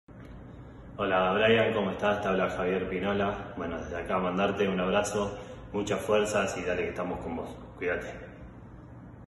Javier Pinola saludó a Braian, le regaló un instante de inolvidable felicidad
Otro de los jugadores de River, el club de sus amores, se sumó al saludo para el adolescente que tiene una difícil prueba con su salud este miércoles en un hospital de la Capital Federal.